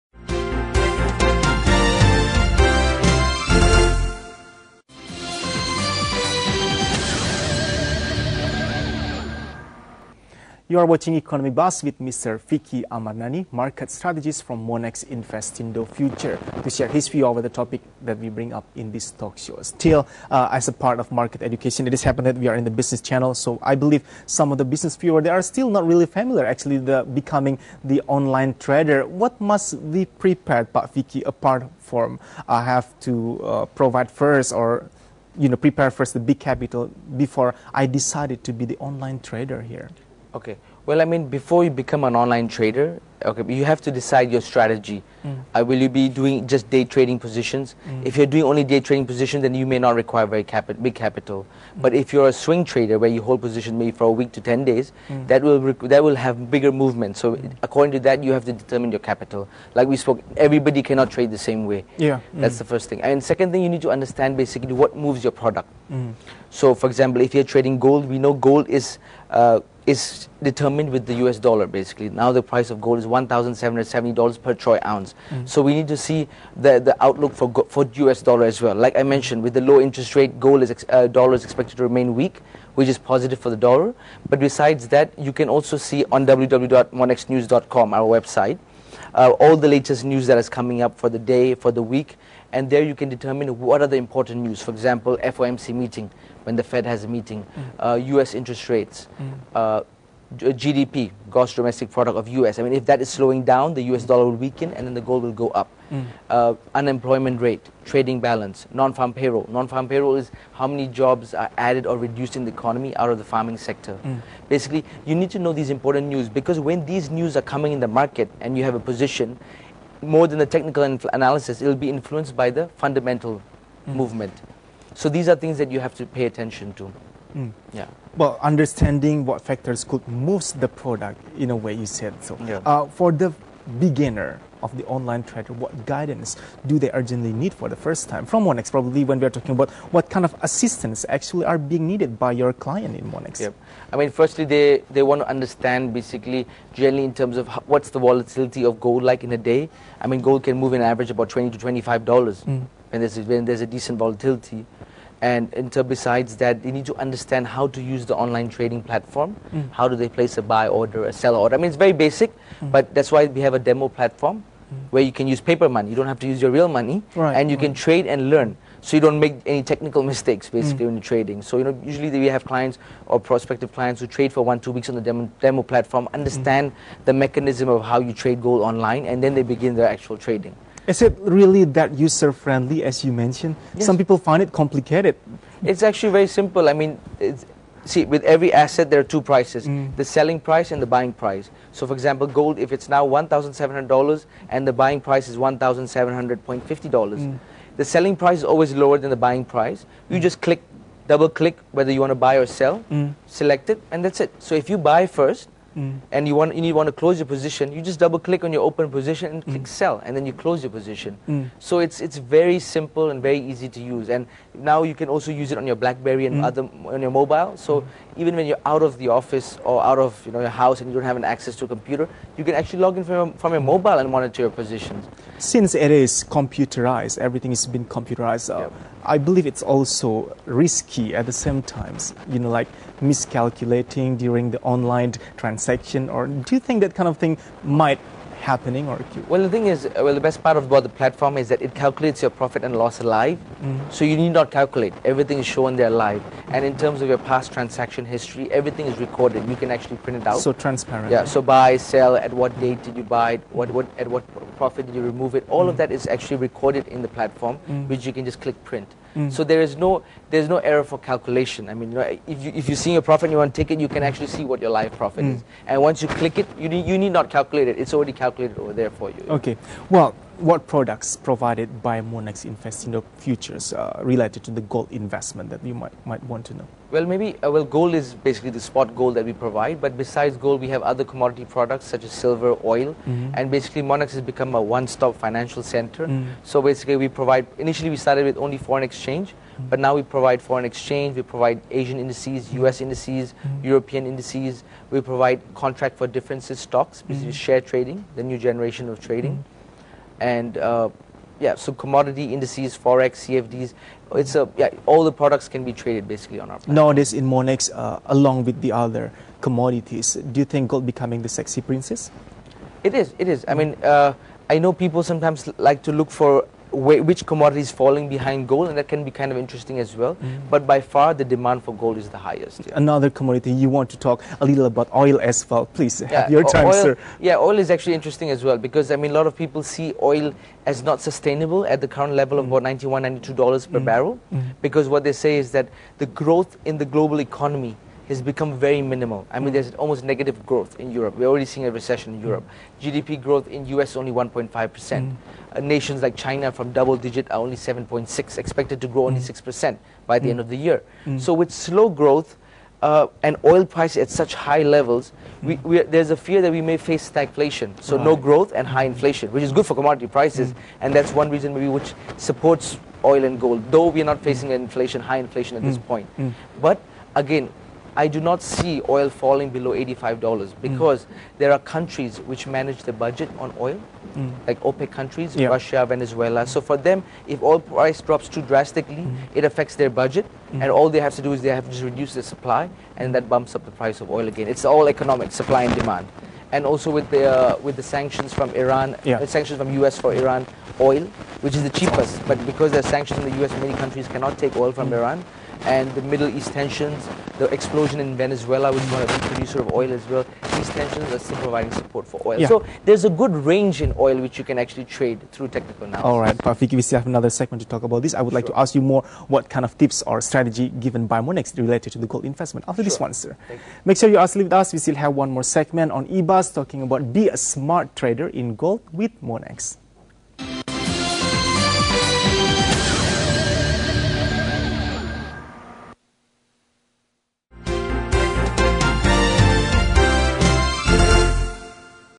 Be A Smart Trader In Gold (Part 3) | Vicky Amarnani monex TV | MNC Business
Be A Smart Trader In Gold With Vicky Amarnani "Monex Market Strategist"